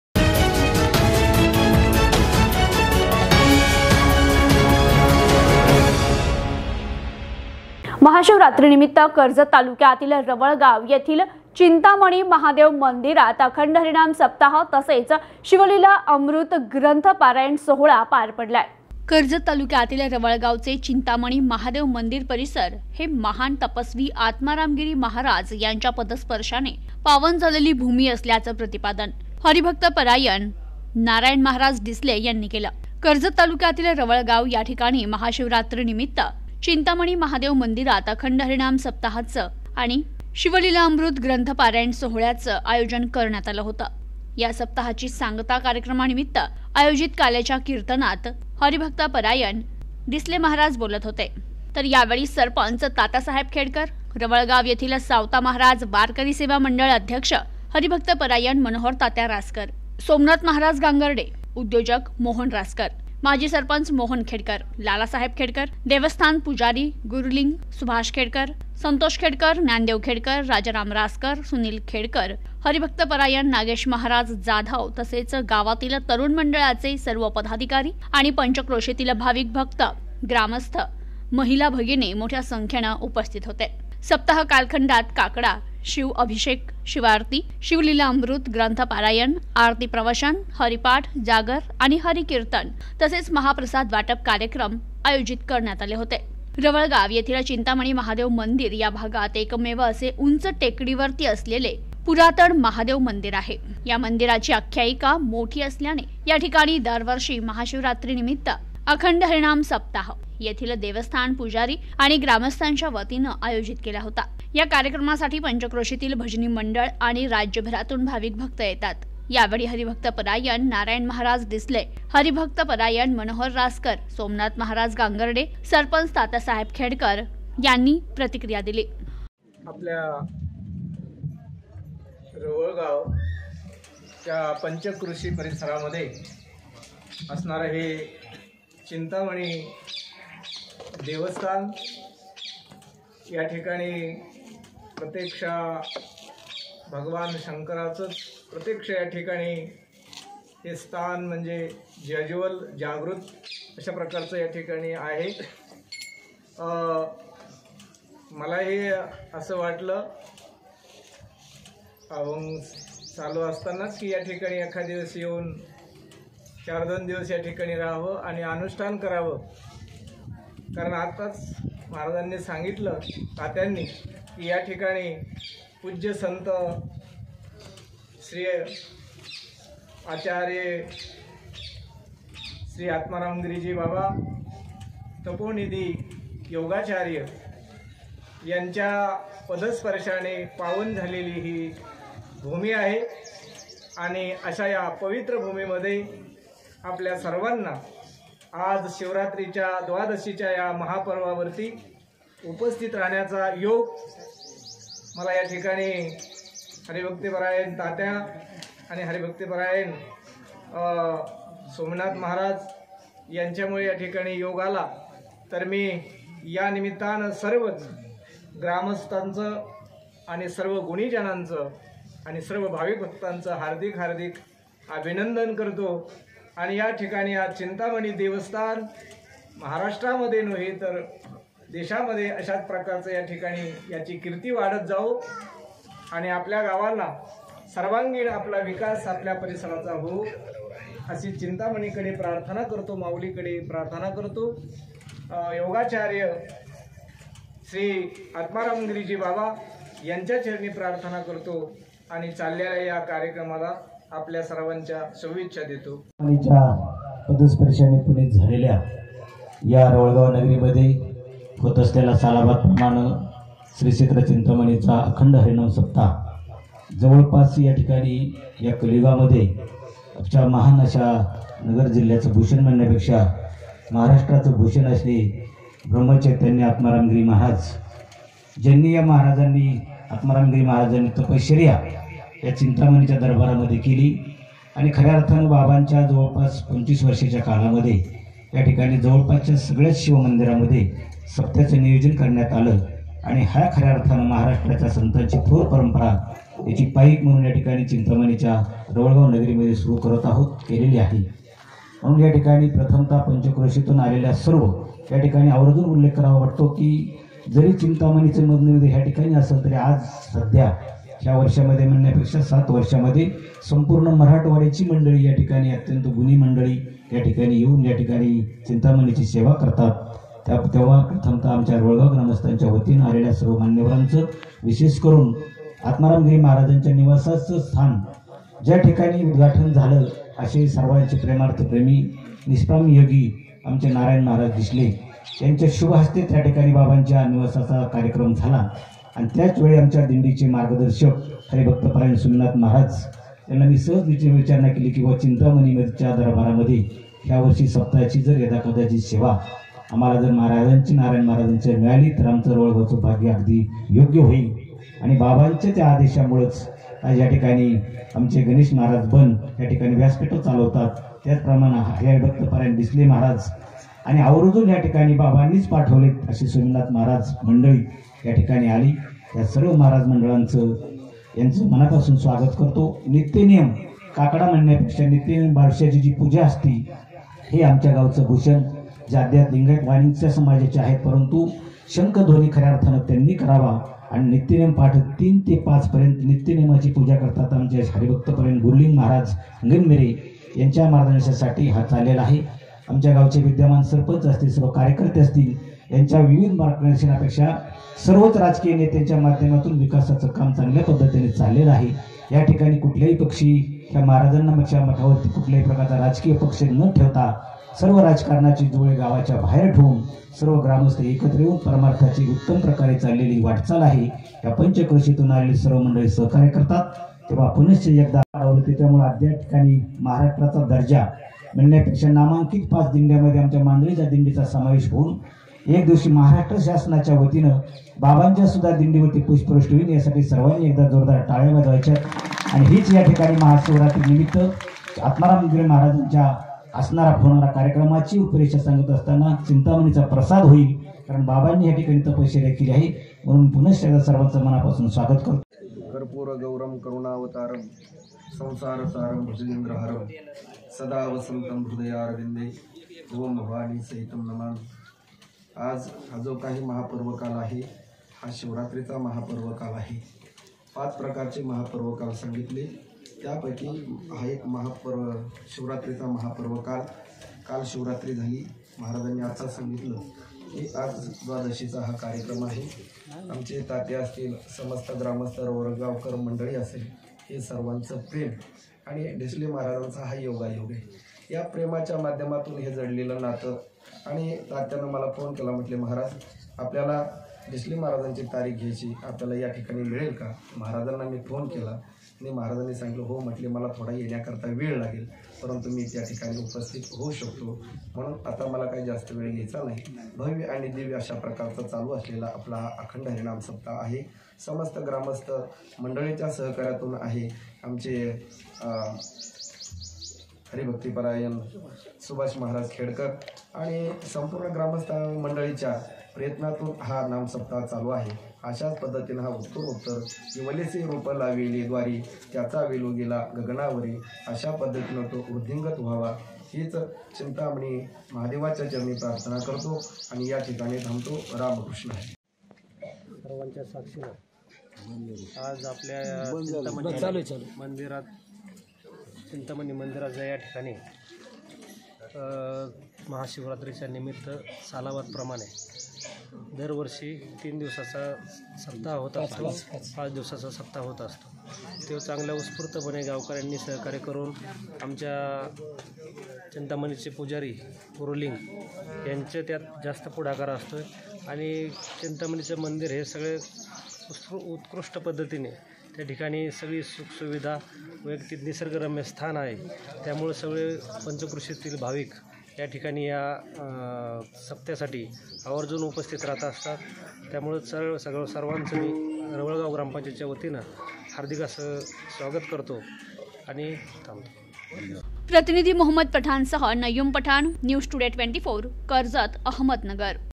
कर्जत : रवळगावचे चिंतामणी महादेव मंदिर परिसर हे पावनभूमी : हभप नारायण महाराज डिसले
महाशिवरात्रीनिमित्त आयोजित अखंड हरिनाम सप्ताहाची सांगता